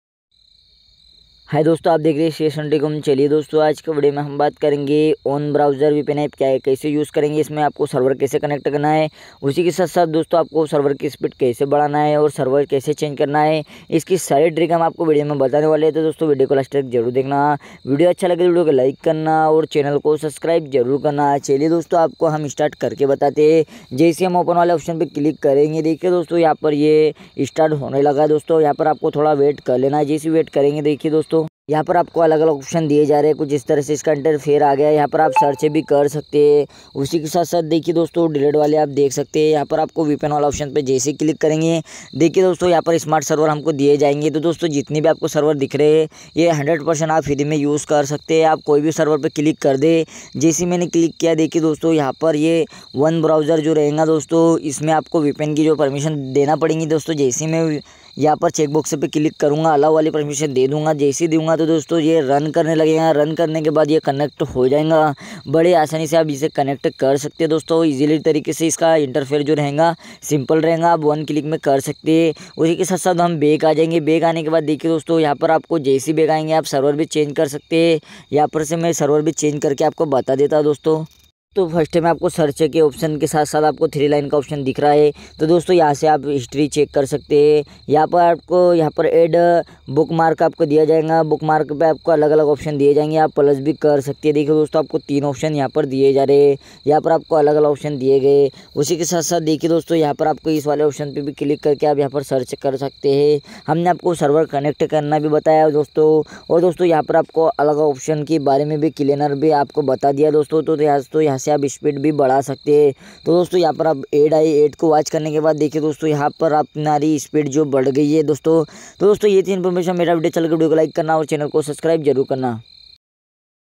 One Browser App kaise use kare ।। how to use one Browser vpn App ।। one Browser vpn App
About video :-
Hello dosto Aaj Ki video me ham baat   karenge one Browser App ke bare.